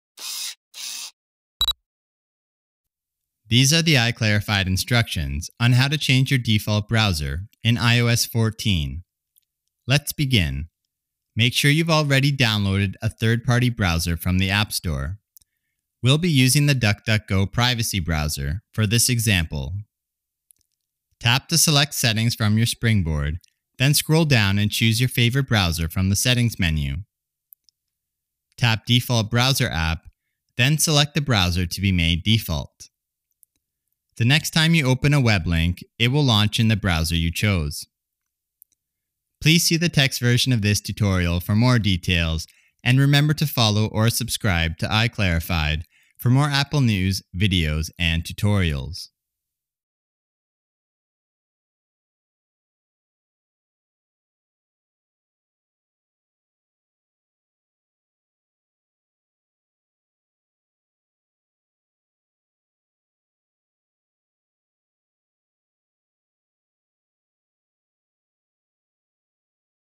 How to Change the Default Browser on Your iPhone
Instructions on how to change the default browser on your iPhone. To perform these steps you must be running iOS 14 or newer.
If you liked this video, please check out Amazon's Deal of the Day!